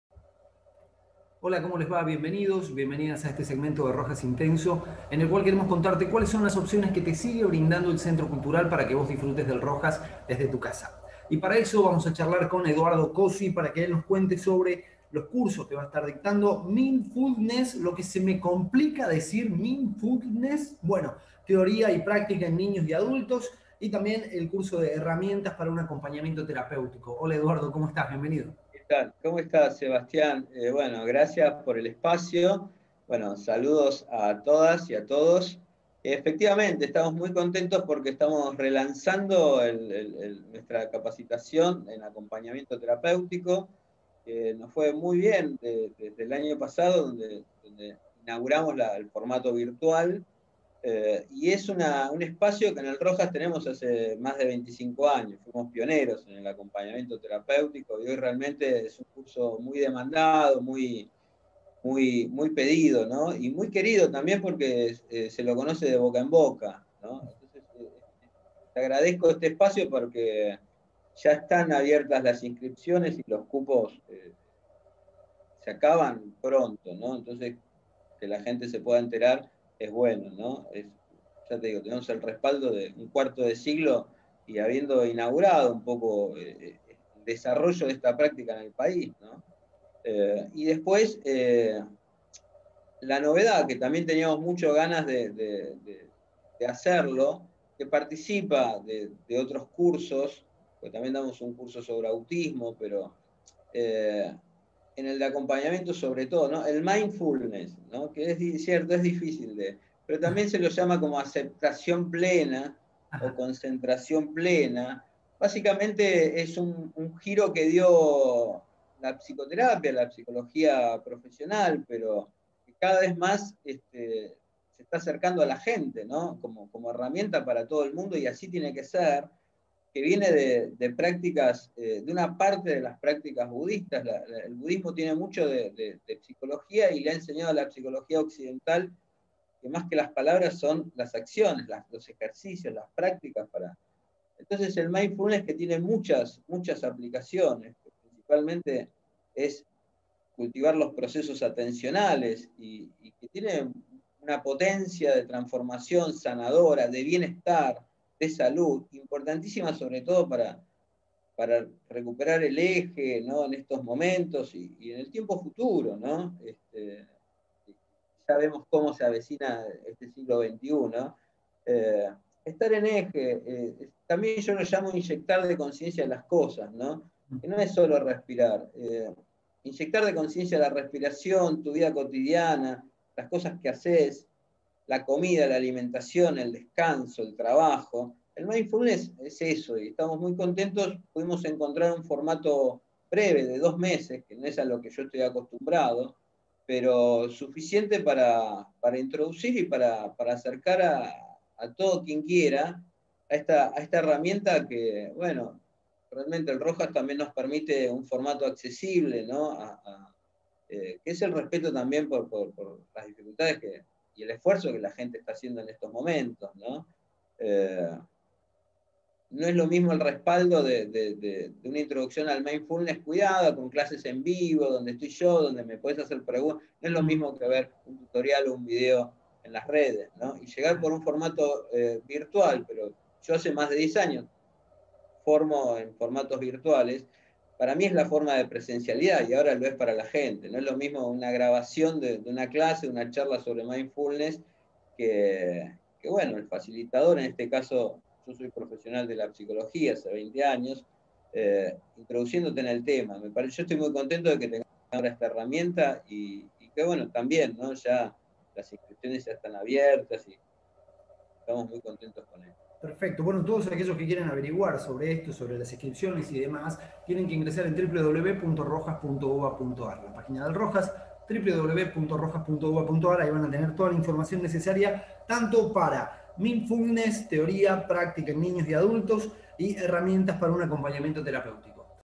Mindfulness, teoría y práctica. Aplicaciones en niños y adultos - Eduardo Cossi
Este curso está destinado a estudiantes y trabajadores del campo social, comunitario, educativo y de la salud y a la comunidad en general, interesados en acercarse al campo del Mindfulness (atención plena) su fundamentación, técnicas y prácticas (ejercicios).
El Mindfulness es en la actualidad la herramientas más eficaz, completa e integral para el bienestar psicológico y a salud en general. Se trabajarán sus orígenes en el budismo, su aplicación psicológica actual. Nos motiva difundir los enormes beneficios del mindfulness y contribuir a que su práctica se expanda con los conocimientos técnicos necesarios y que se haga de manera consciente, ayudando a la vida personal de los interesados.
Eduardo Cossi es Doctor en Psicología (UBA). Director de tesis: Phd. Ernesto Laclau. ESSEX, London. UK .Postgrados:Diplomatura en DDHH DESC (UNLA). Secretaria de DDHH. Ministerio de Justicia y DDHH de la Nación. 2011. Programa de actualización en Psicoanálisis Lacaniano.144hs (UBA). Programa de Actualización de Capacitación en Procesos Psicológicos Básicos y Metodología de la Investigación en Psicología”. Programa FOMEC. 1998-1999 (UBA).

Inicio: 15 de junio 2021 | Duración: 8 semanas